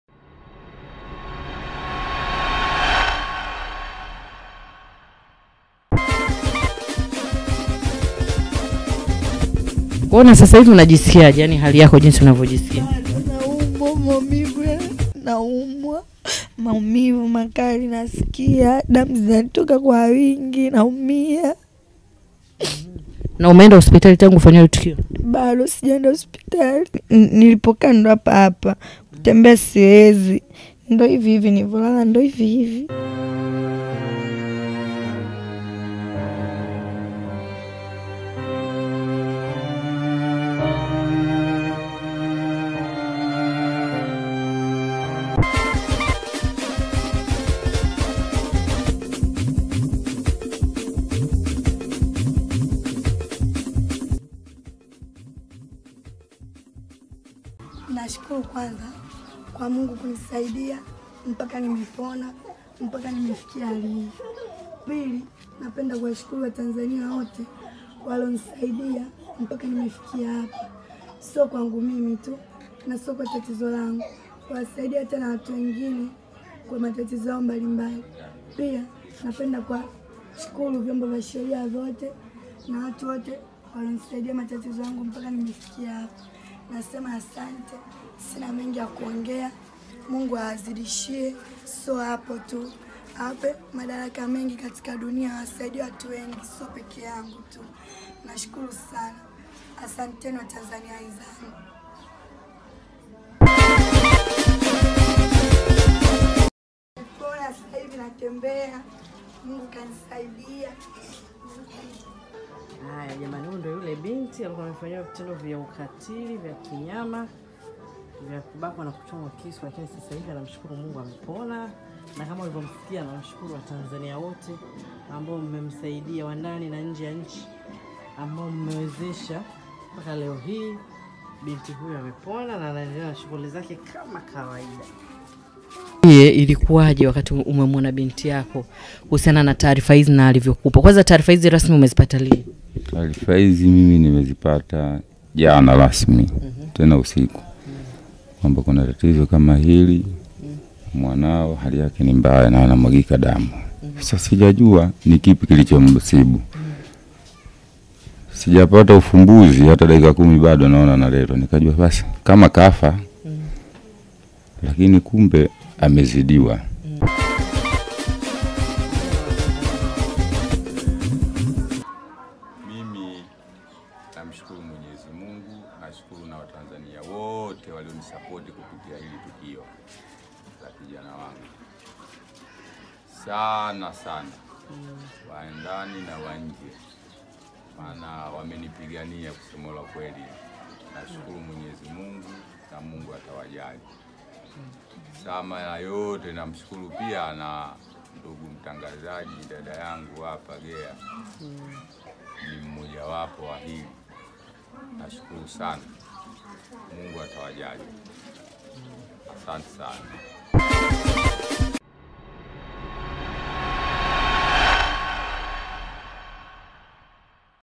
BINTI ALIYEBAKWA NA KUCHOMWA KISU SEHEMU ZA SIRI AMEPONA/AWASHUKURU WOTE MLIOMSAIDIA
Nilirudi kwa yule binti aliyefanyiwa vitendo vya ukatili ambapo alibakwa na kuchomwa kisu sehemu zake za siri!Bintu ametoka hospitali na amepona sasa hivi anatembea vizuri anawashukuru watu wote mlioweza kumsaidia.Mtizame binti mwenyewe akitoa shukurani zake hapa